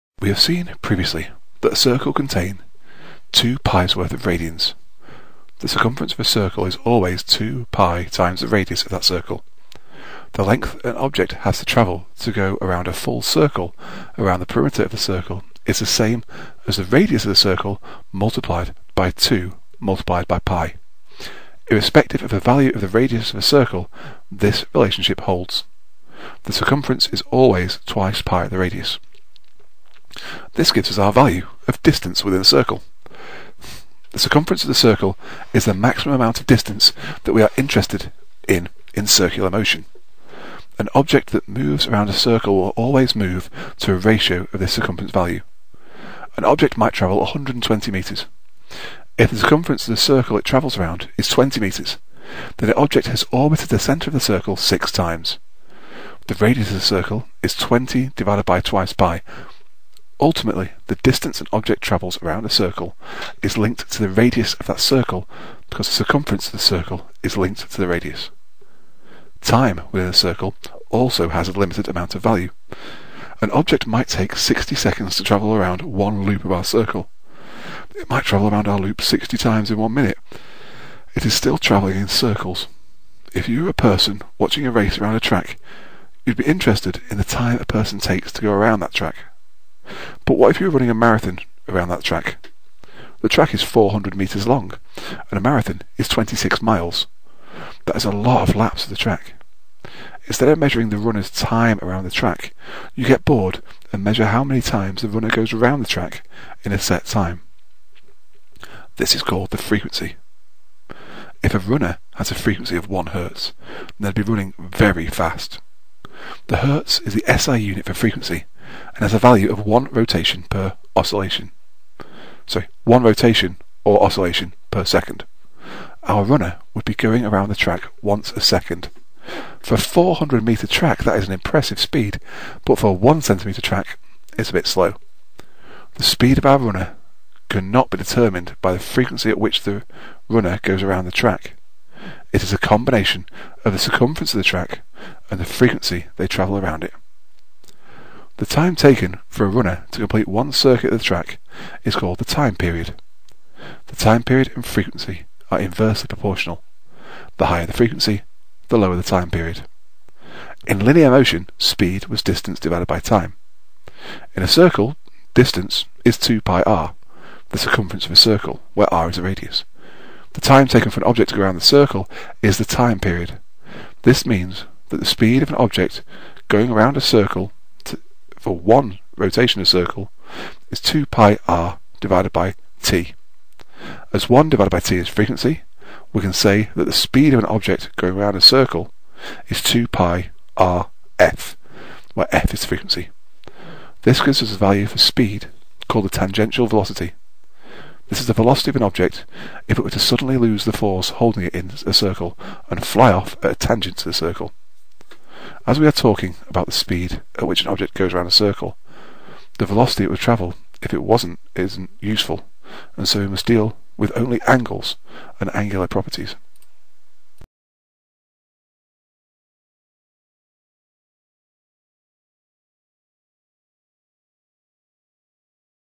4 Translating linear to circular
Part of my "Advanced Physics Tutor" website. This video describes the similarities and differences between linear and circular motion.